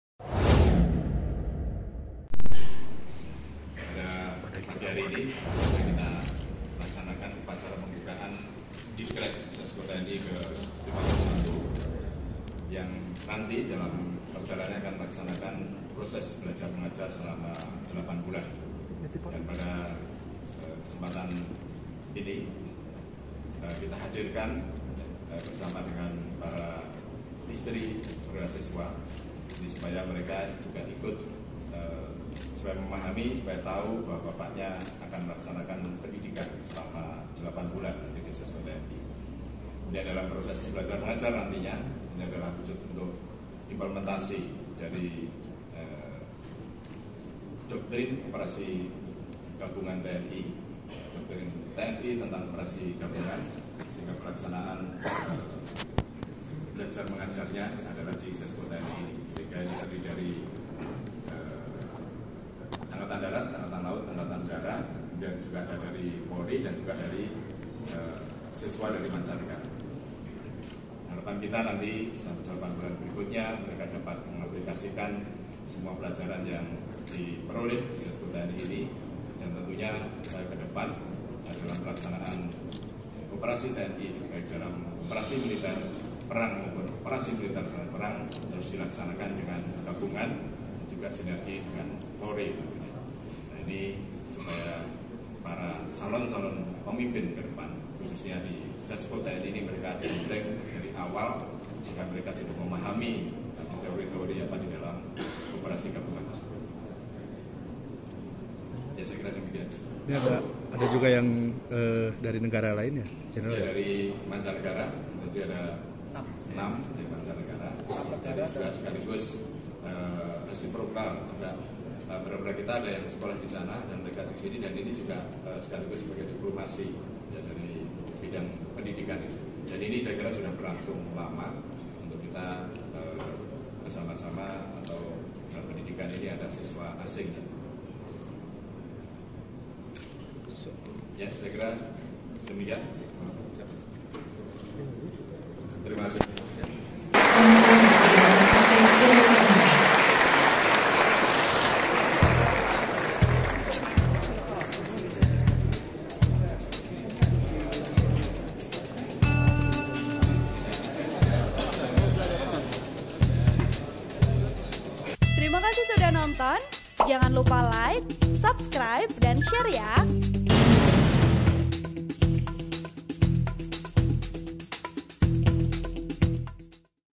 Tujuh Perwira Siswa Negara Sahabat Ikuti Pendidikan Operasi Militer Perang Bersama di Sesko TNI
Penulis: Nazmi Abdurrahman | Editor: Seli Andina Miranti | Video Editor: Edwin Tk